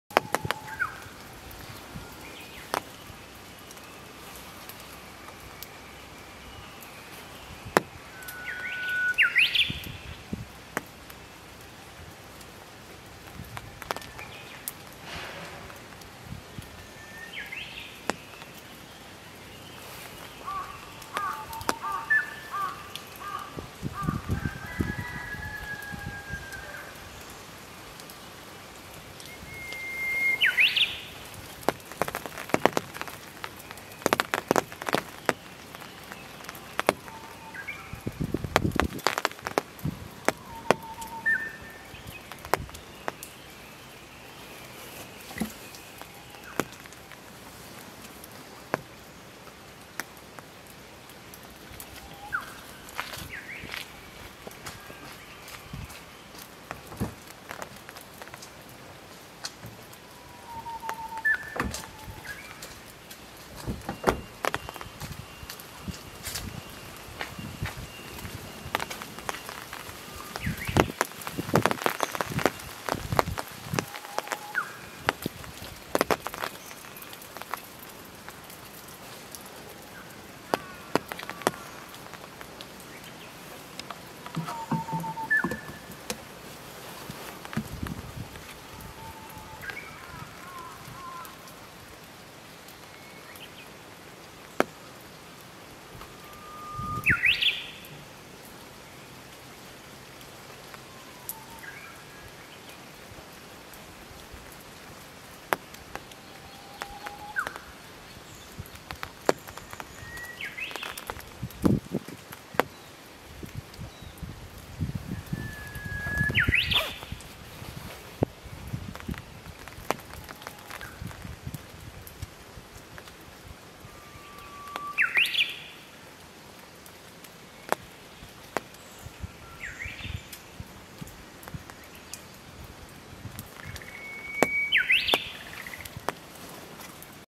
ウグイス の サエズリ
ウグイスが15m先で鳴き始めた。